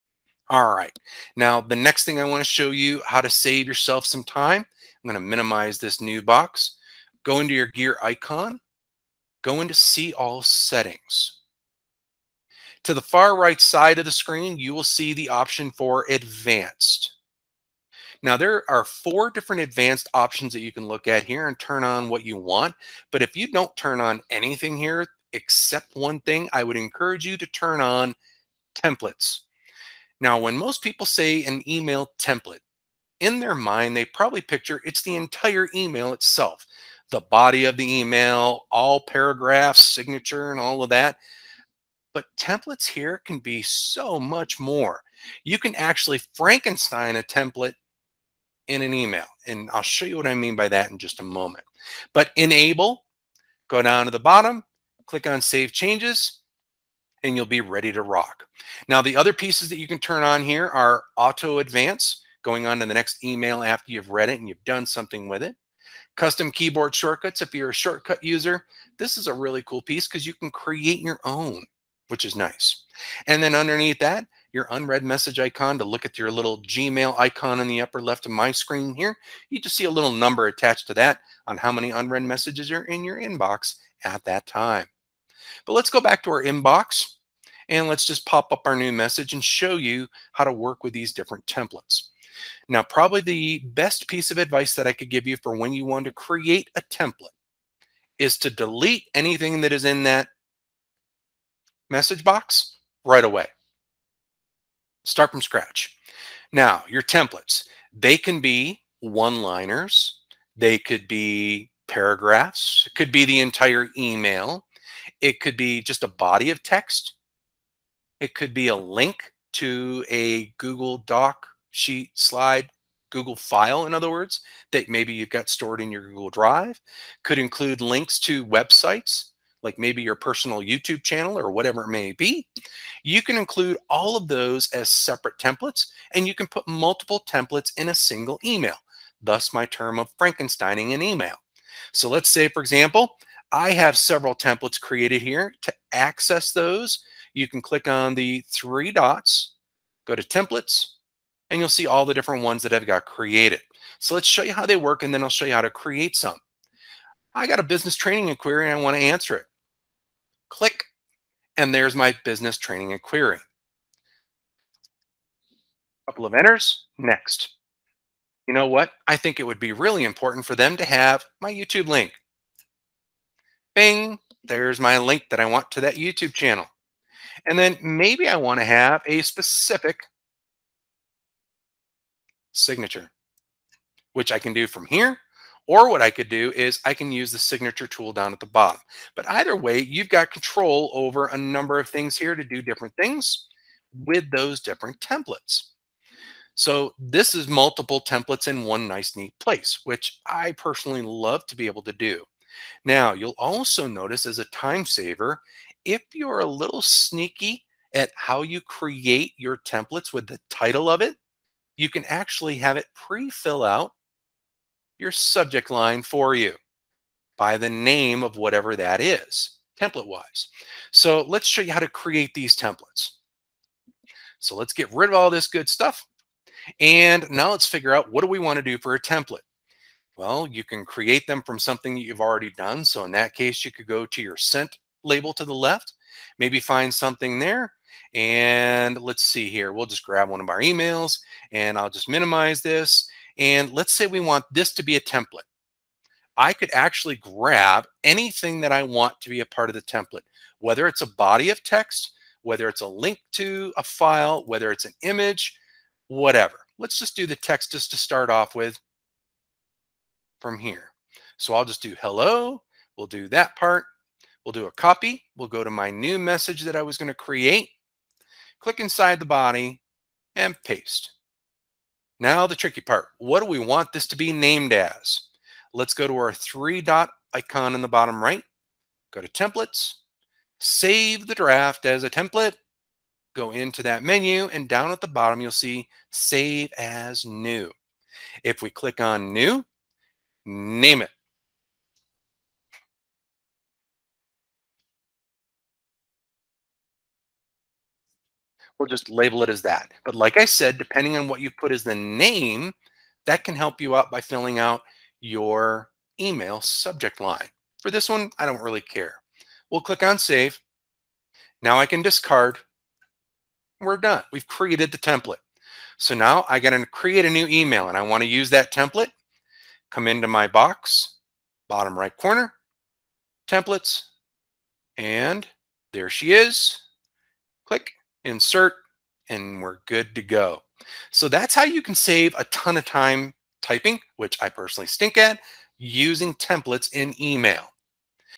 How to Save Time Writing Emails With Gmail Templates
Today, you're going to learn how to save time writing emails with gmail templates! Create templates to create quick emails then! You can add text, multiple bodies of text, links, images, and Drive file links. Hope this helps!